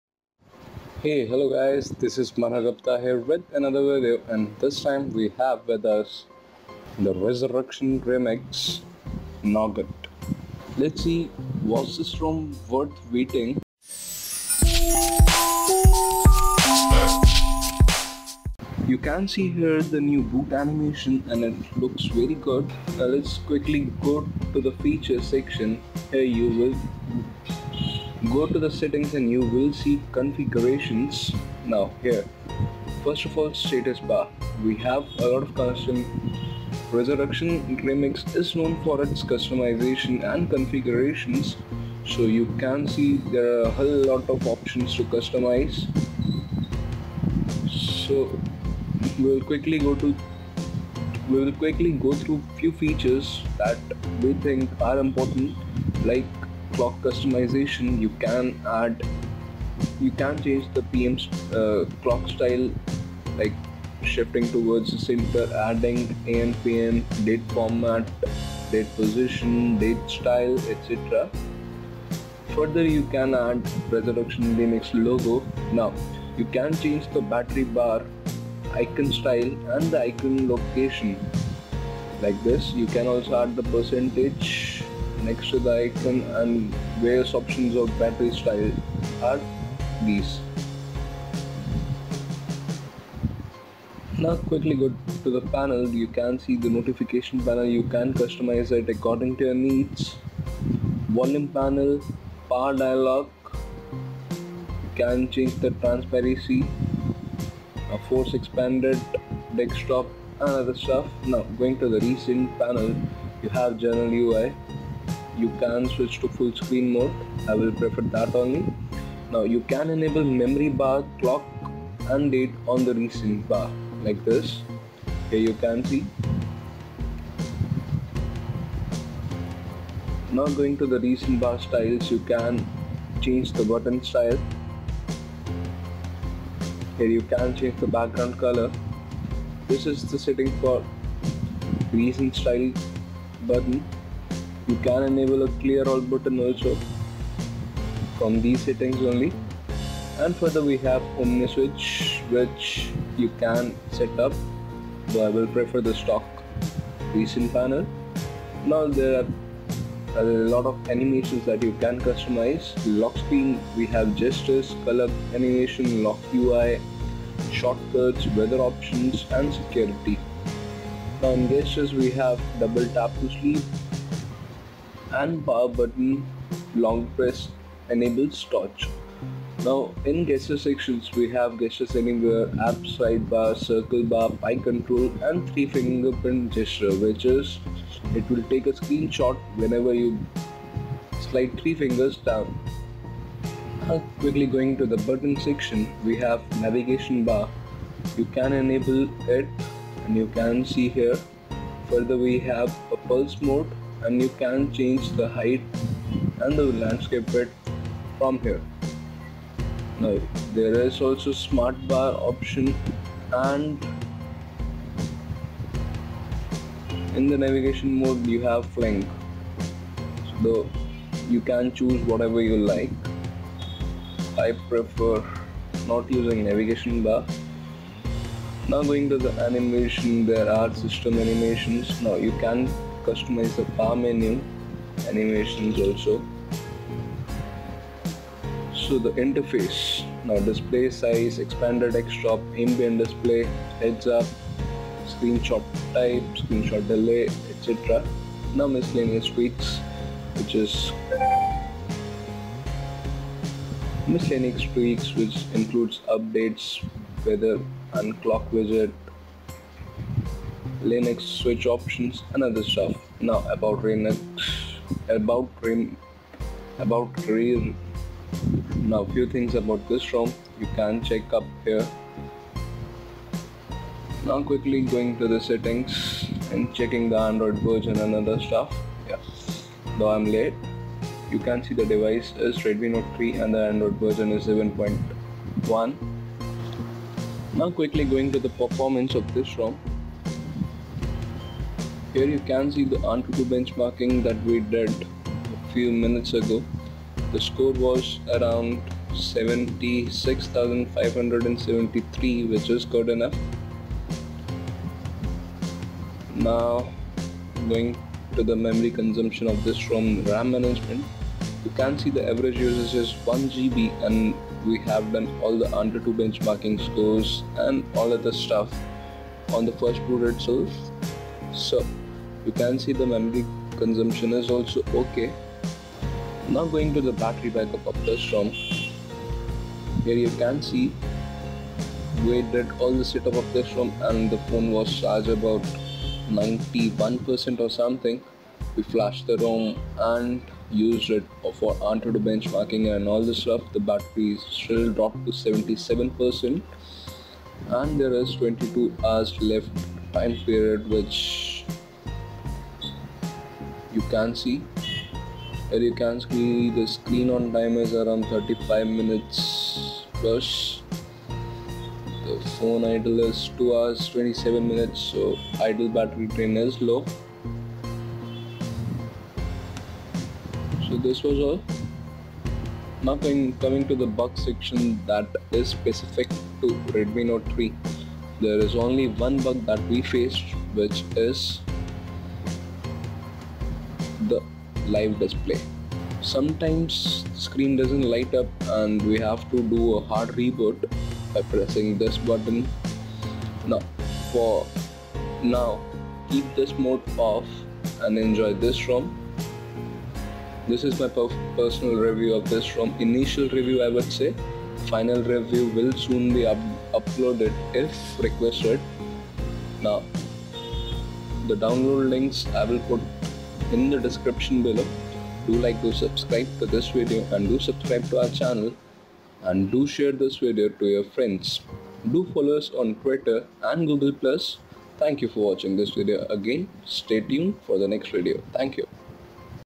Official Resurrection Remix Nougat 5.8 Features and Review Worth The Wait ! | Redmi Note 3 | VoLTE
Resurrection Remix the ROM has been based on CM,slim.omni and original Remix ROM builds, this creates an awesome combination of performance, customization, power and the most new features, brought directly to your Device
Many things that in previous versions were tweaked with mods, are now included by default in the ROM so, please enjoy!